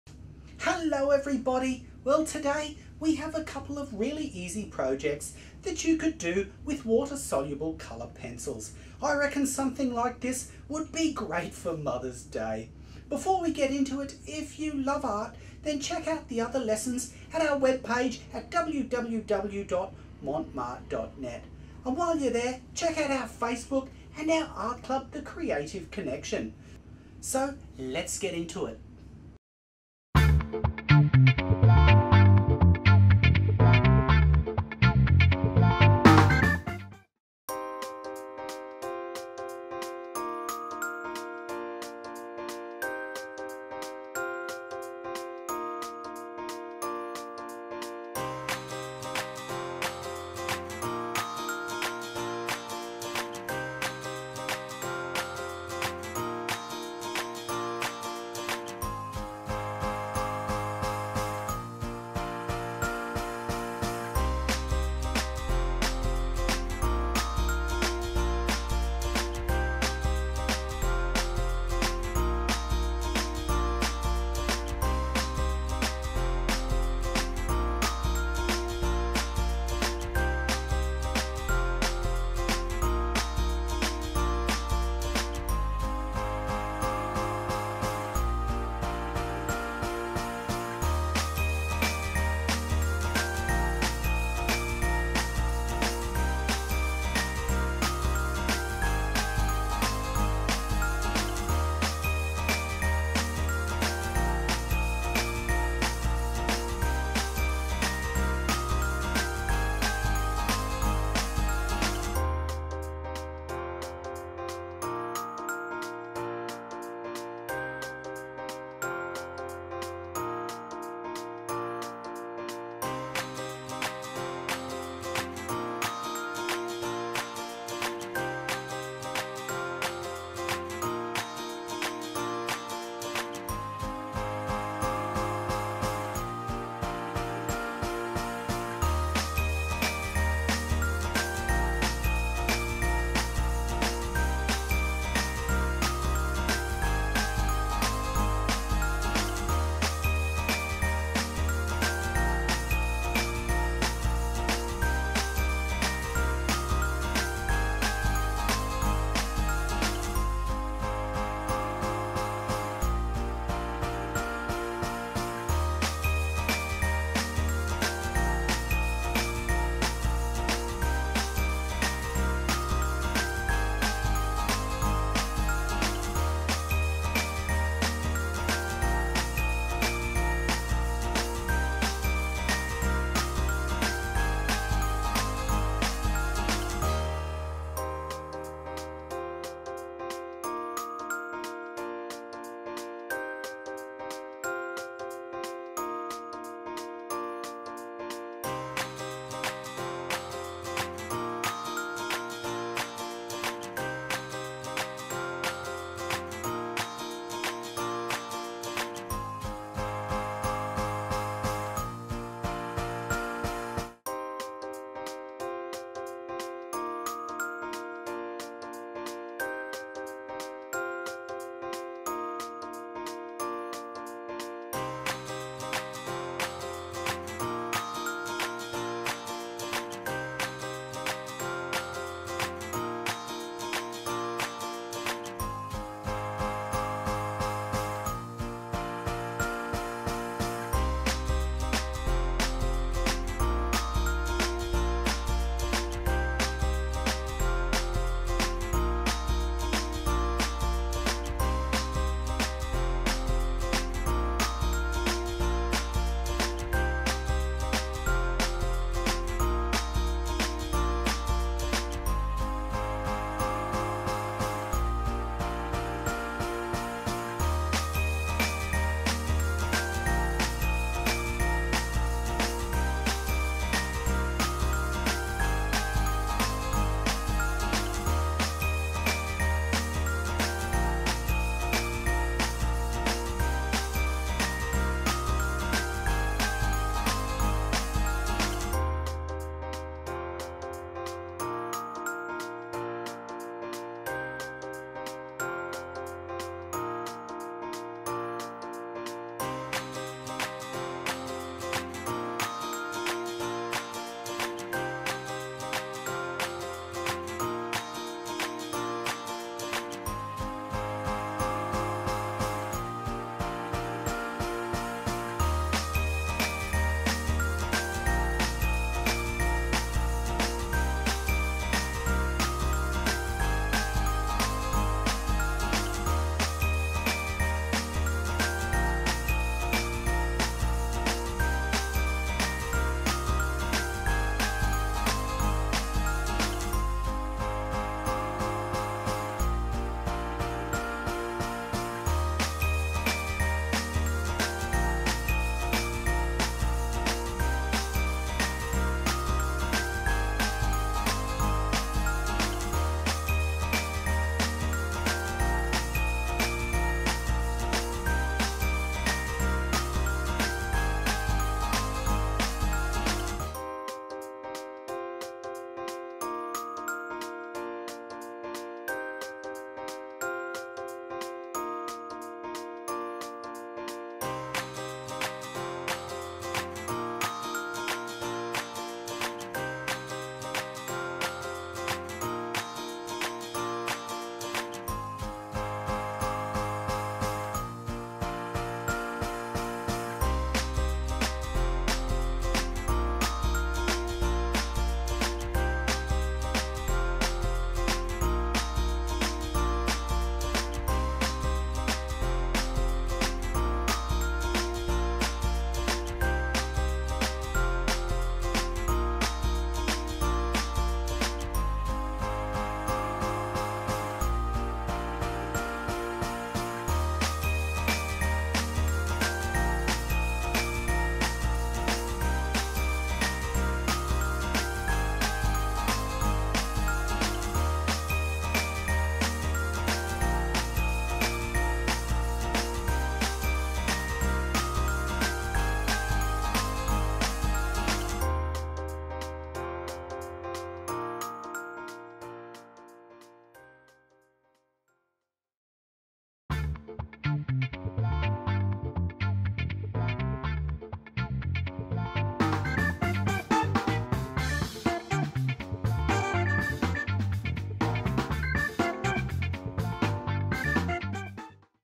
Art Lesson: How to paint Butterflies with Watercolour pencils
Learn how to paint butterflies with watercolour pencils!